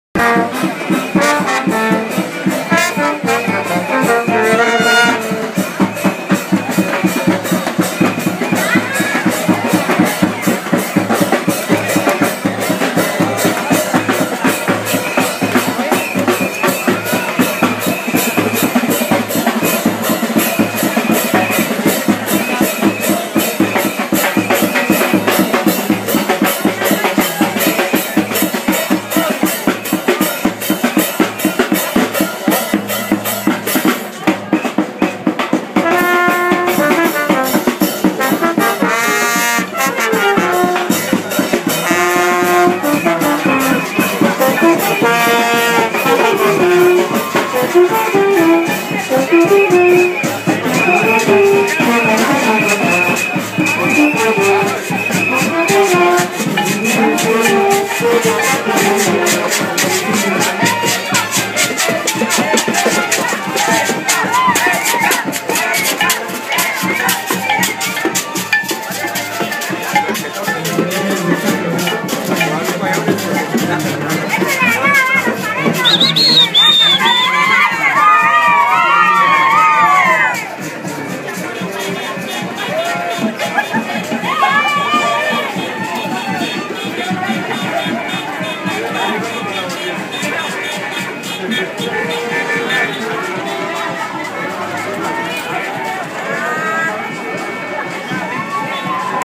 OTRA VUELTA CON LAS REINAS.mp4
Desfile de carrozas y reinas en las Ferias de San José de Pare 2013...